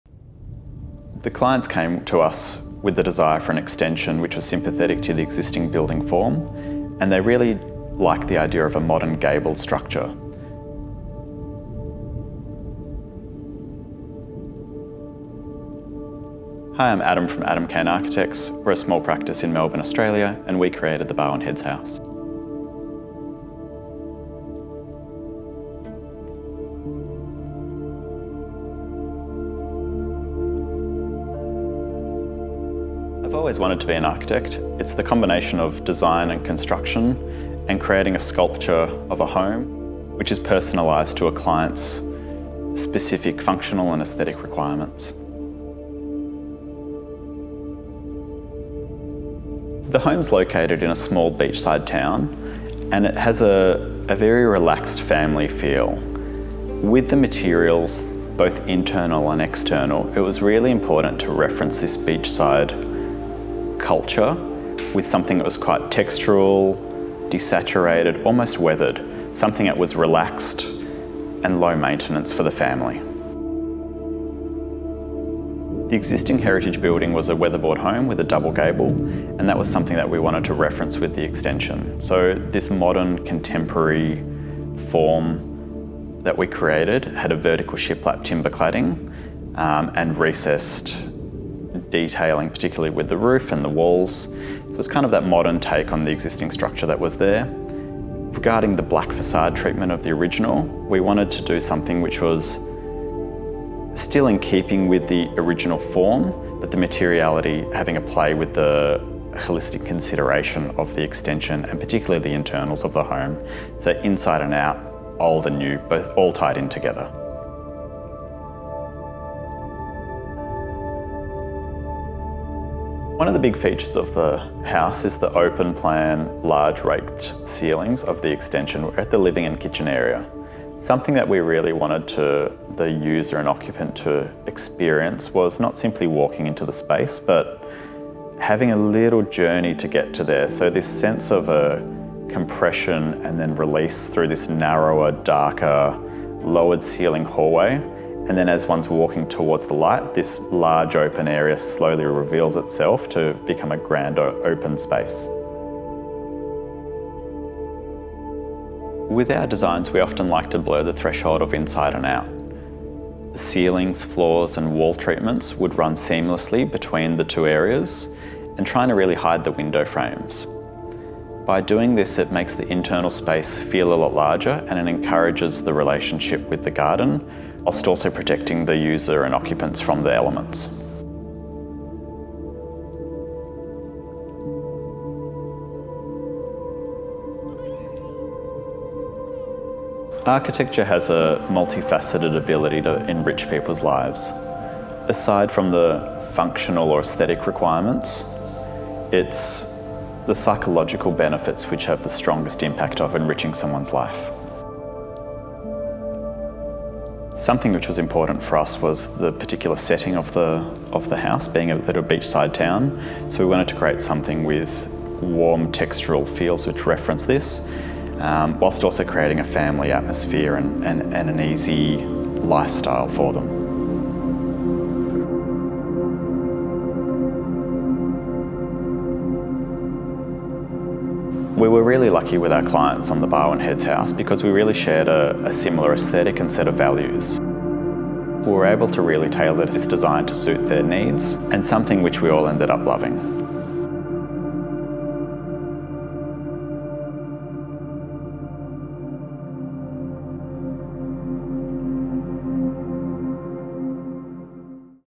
House in Australia designed by Adam Kane Architects: Interview with Architect | ARCHITECTURE HUNTER
Adam Kane Architects explains the design of this coastal-inspired house, influenced by the relaxed lifestyle of Barwon Heads. The renovation seamlessly blends old and new elements, ensuring a strong connection between indoor and outdoor spaces. Upon entering, the bright interior with original lining board ceilings and period details beautifully contrasts with the new charcoal floorboards. With its interplay of light and dark, solid and void, and unique materiality, the home offers a truly special experience for both residents and guests to enjoy for years to come. Dive into In Loco with Adam Kane Architects!

Project: Barwon Heads House
Architecture: Adam Kane Architects
Location: Barwon Heads, Australia
Film: Architecture Hunter & Mavix
Cover image: Timothy Kaye